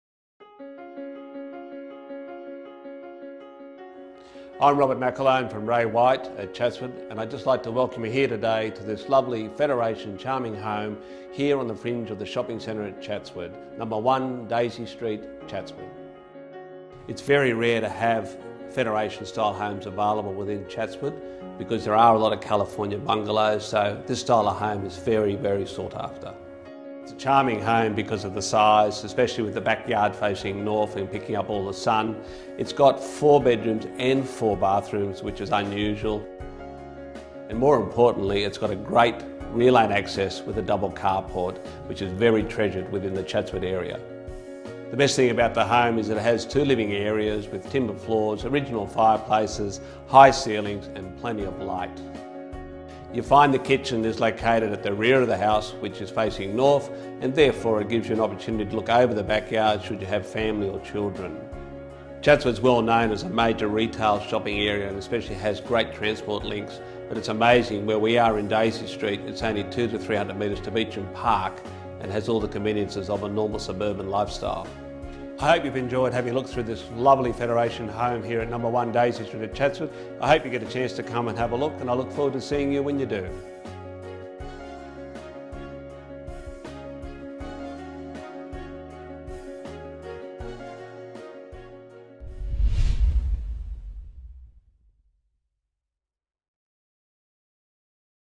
1 Daisy Street, Chatswood
TIMELESS ELEGANCE

This charming 1920's Federation residence offers a dress-circle location on one of Chatswood's highly sought after Flower Streets'. Graced with child-friendly gardens and a coveted north to rear aspect, this home showcases a breathtaking blend of classic vintage character alongside sleek modern finishes.

Single level full brick construction on original sandstone foundations
Picturesque period faade on a level 613m (approx) block framed with hedges
Four large bedrooms, including teenagers' retreat or home office with private entry
Four bathrooms including two en-suites, main with shower over corner bath
Beautiful period features include lead lights, fireplaces with tiled surrounds, soaring patterned ceilings and hardwood floors
Sundrenched gas kitchen with stainless steel appliances, snack bar and meals area
Open plan living and dining , formal lounge with bay window
Air conditioning, Rheem gas hot water
Double carport with remote auto door, access from rear lane
Short walk to Beauchamp Park, business, shopping centres, rail, buses and schools.
Ultra convenient location only 20 minutes to the CBD